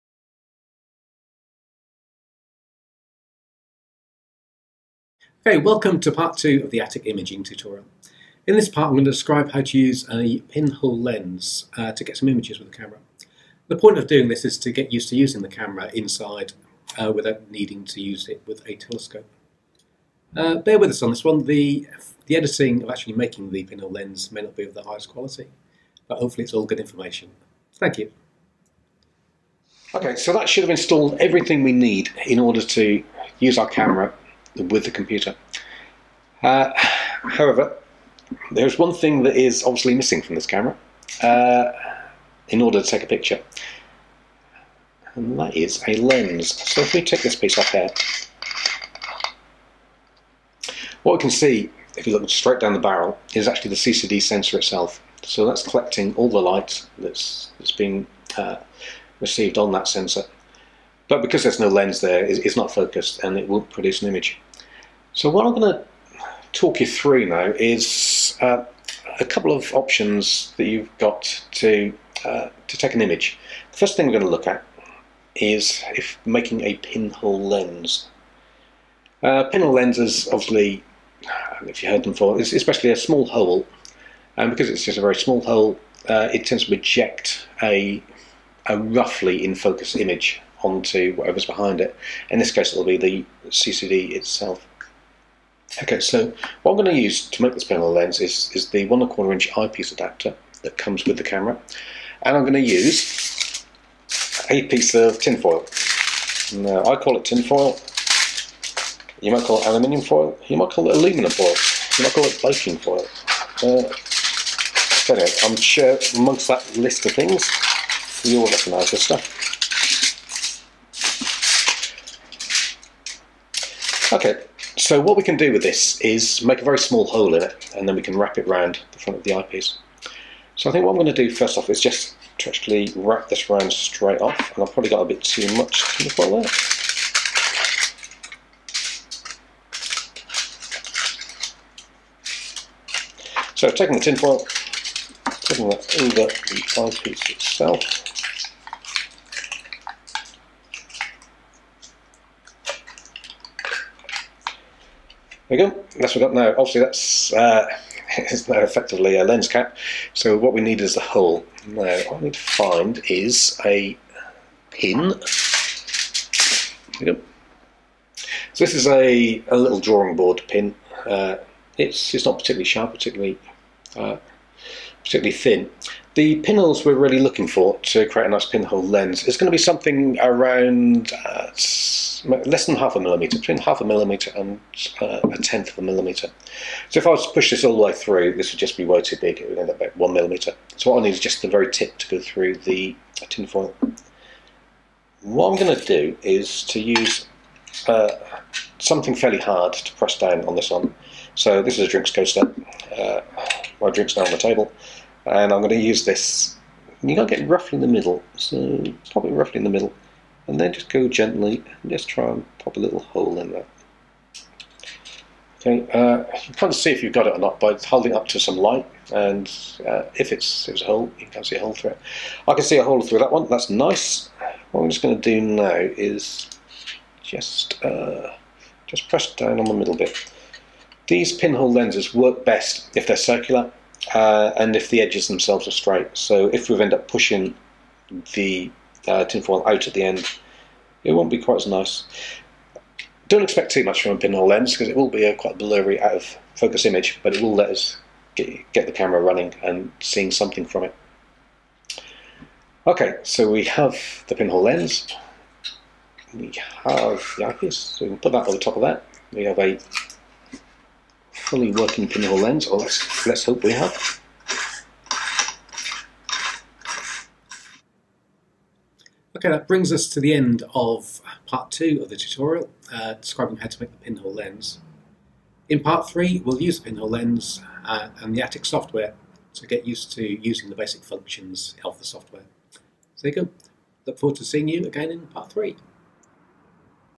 Atik Getting started tutorial, Part 2
Atik getting started tutorial.  Making a pin hole lens to use with the camera.